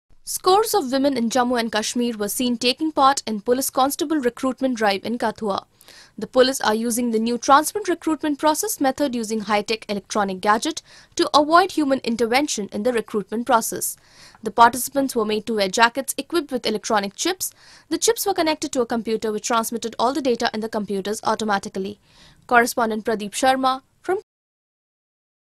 जम्मू कश्मीर में पुलिस में भर्ती के लिए महिलाएं सक्रिय | Women take part in Police recruitment
जम्मू कश्मीर में पुलिस में भर्ती के लिए महिलाएं सक्रिय कठुआ में पुलिस कांस्टेबल की भर्ती में बड़ी संख्या में राज्य की महिलाओं ने भाग  लिया भर्ती के दौरान पक्षपात रोकने के लिए अत्याधुनिक उपकरणों की मदद ली गई l